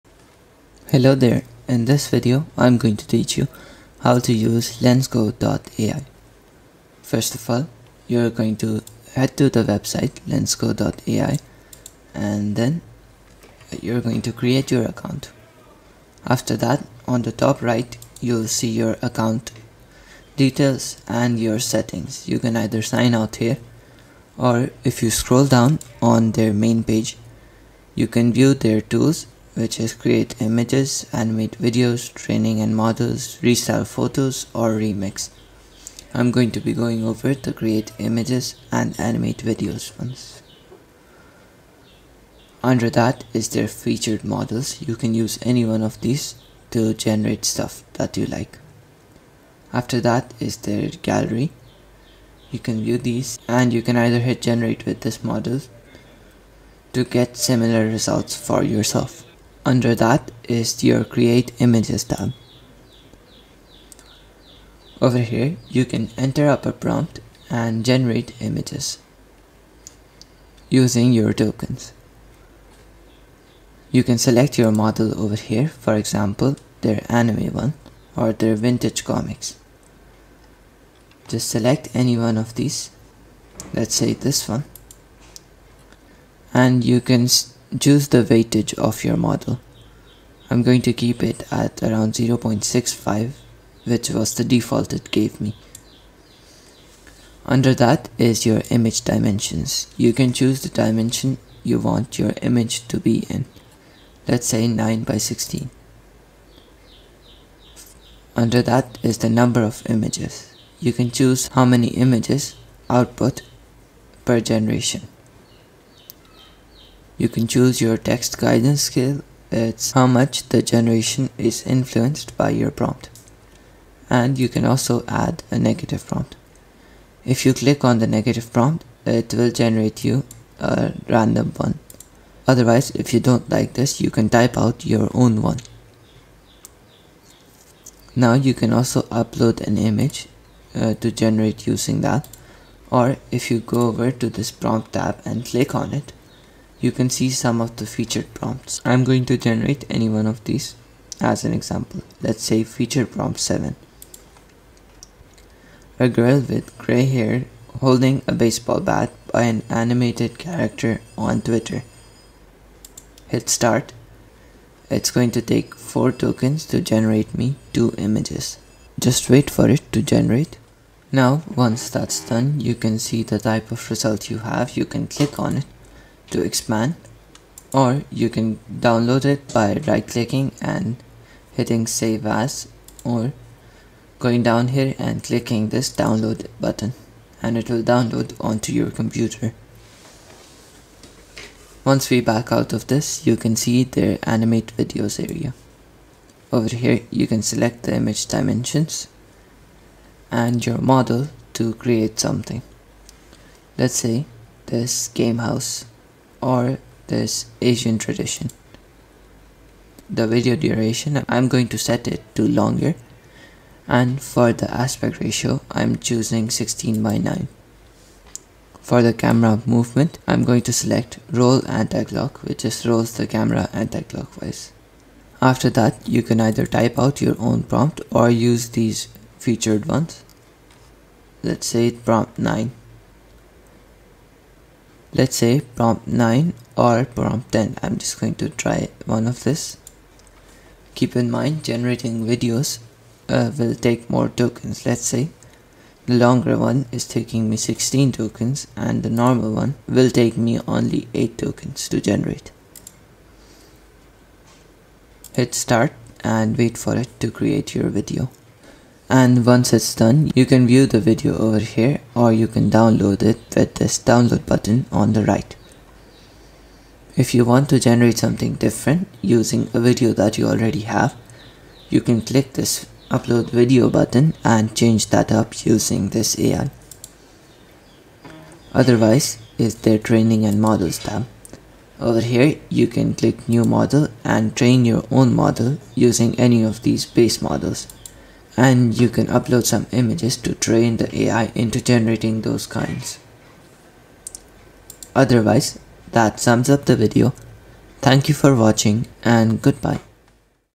LensGo AI Tutorial
In this quick video, you will get a full LensGo AI prompts tutorial and learn how to make animation with AI.